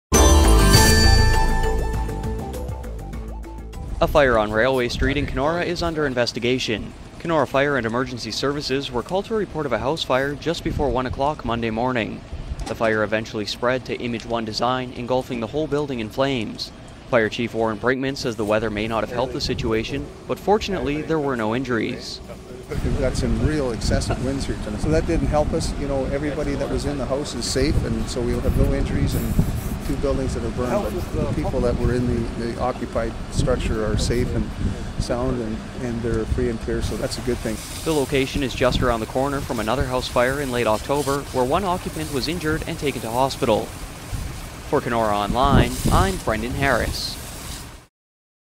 Fire on Railway St.
Kenora Fire and Emergency Services respond to a house fire on Railway St.